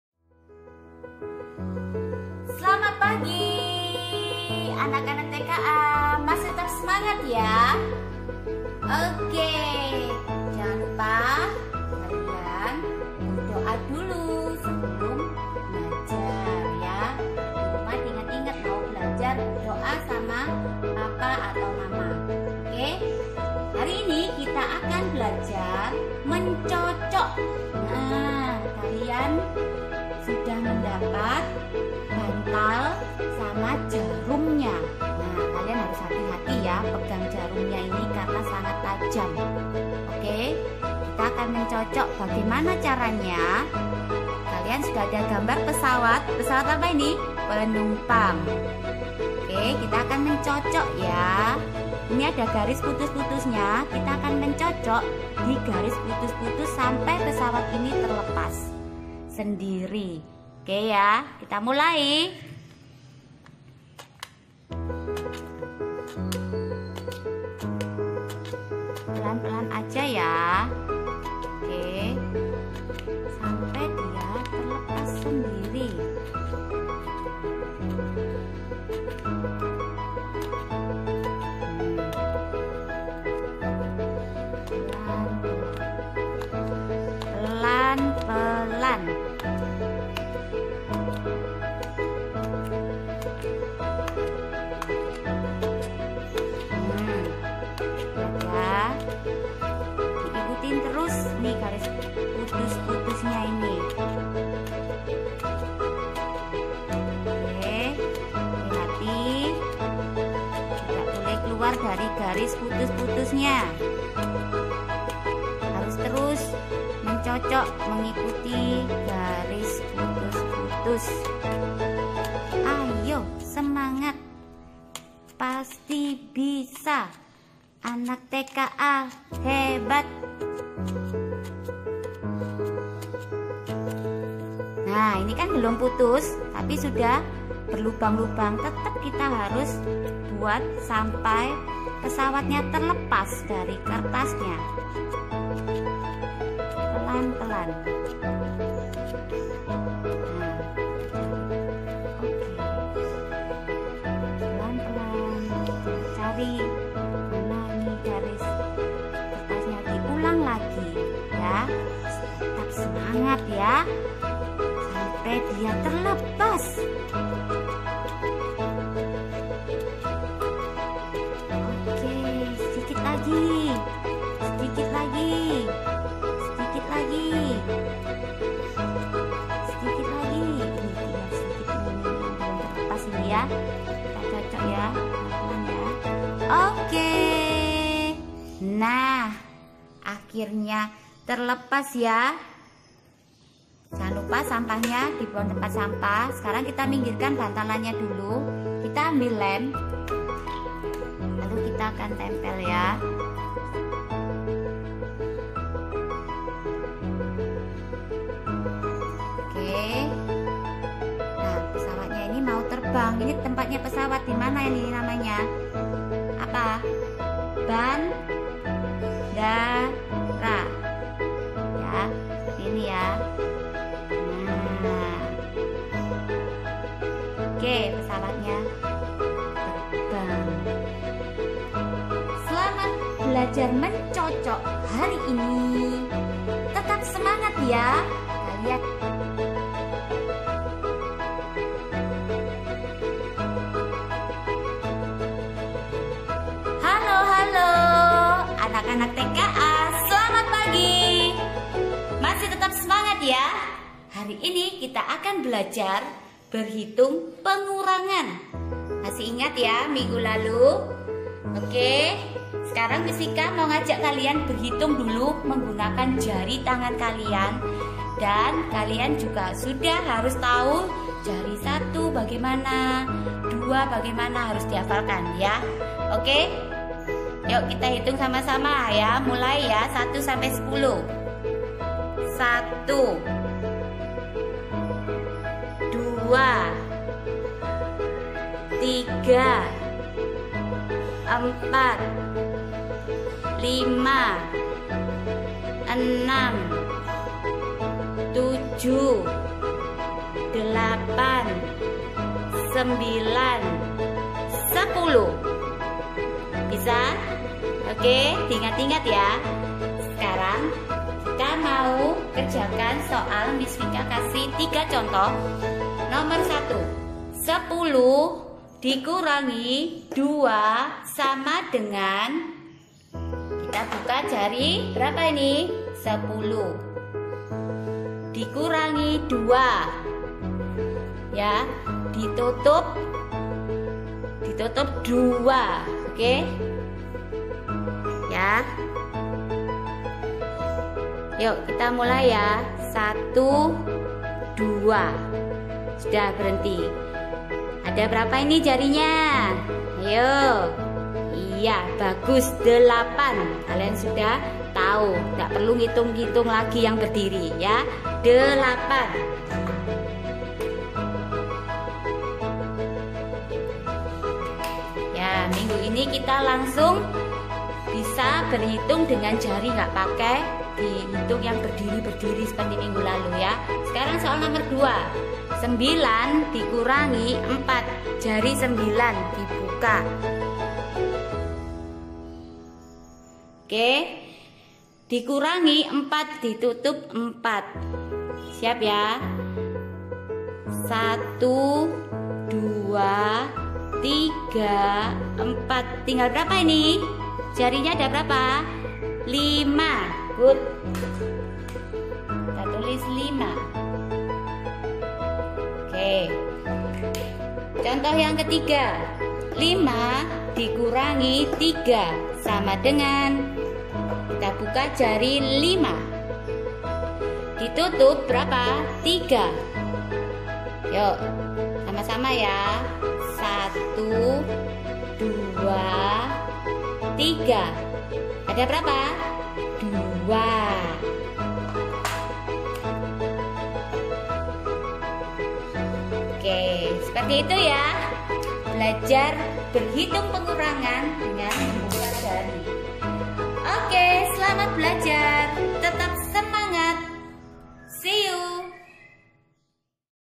Ms. Vika's Kindergarten Class : Home Learning
Kegiatan ini melatih kognitif dan motorik halus pada anak.